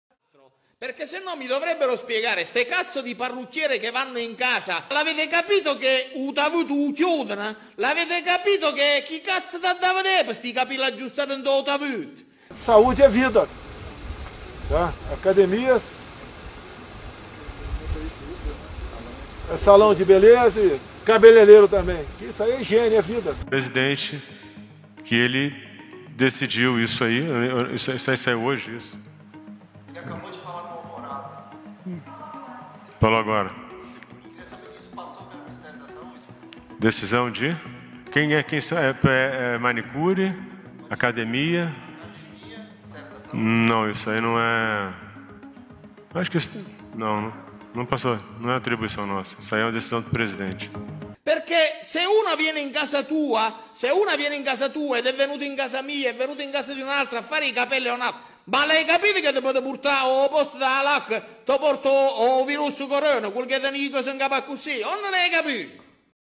Reabertura dos salões de beleza vai gerar mais mortes por coronavírus.
Fonte: Levante Popular da Juventude